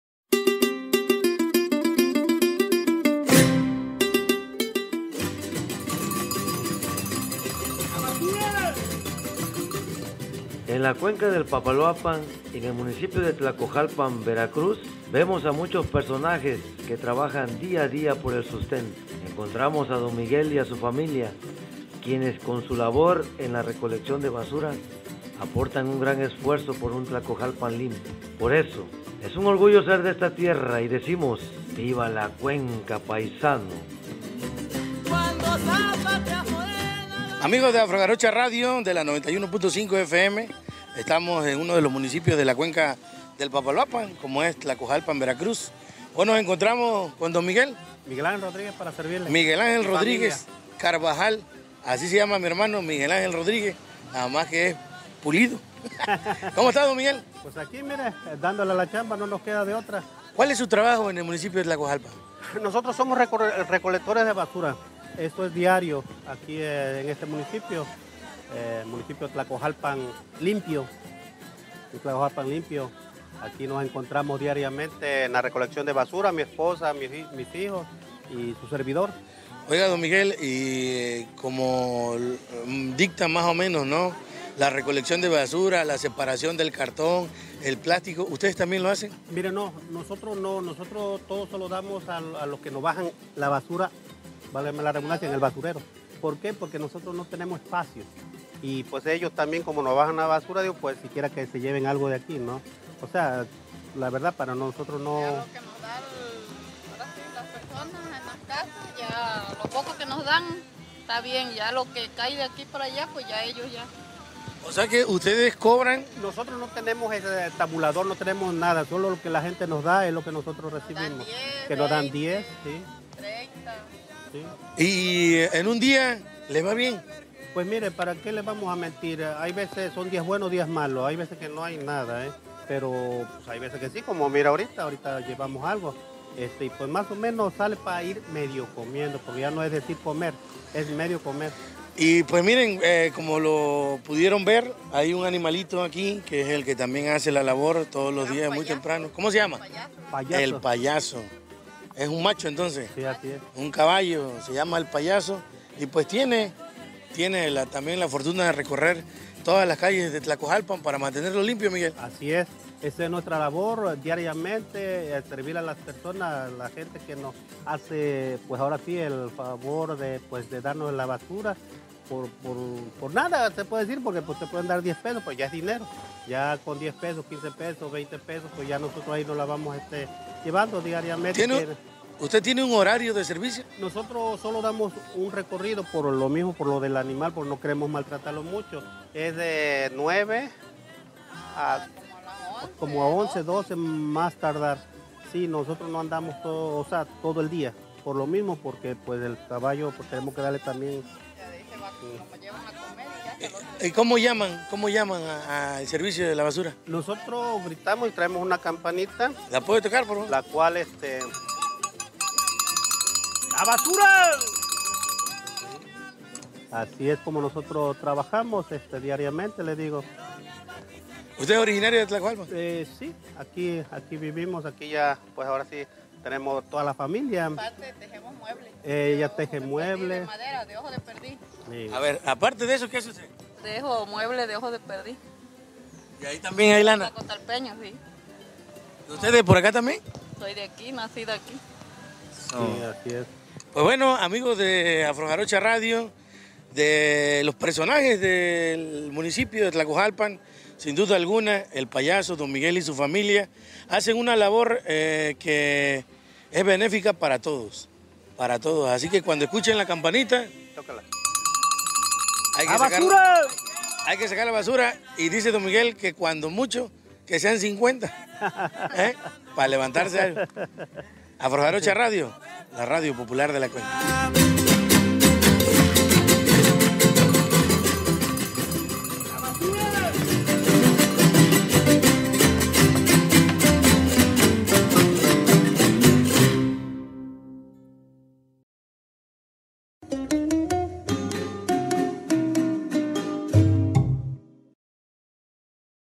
"PERSONAJES DE TLACOJALPAN"
En la cuenca del Papaloapan la gente trabaja día a día para el sustento, en Tlacojalpan, Veracruz Don Miguel y su familia suman esfuerzos por un Tlacojalpan limpio.